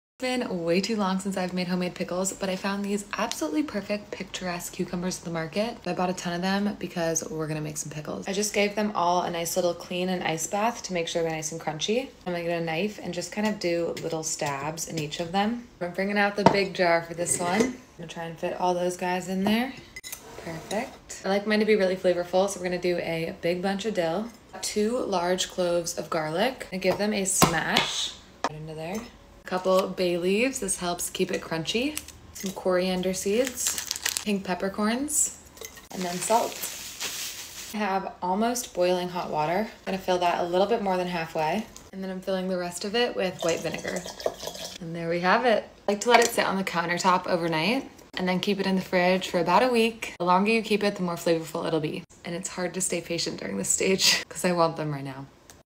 pickles baby 🥒
Ingredients:- 6 chunky cucumbers- 2 cloves garlic- large handful dill- 2 tbsp peppercorns- 2 tbsp salt- 2 bay leaves or grape leaves- 1.5 cups white vinegar- 2 cups hot water- optional: 1 tbsp maple syrup or raw honey- optional: jalapeñorecipe:1. clean cucumbers and place in ice bath for 15-20 mins2. stab them OR cut them into spears or lil chips3. place them in a large jar and add dill + seasonings4. smash garlic cloves and add in5. top with hot water and vinegar6. place on the counter overnight and then in the fridge for about a week or longer7. enjoy!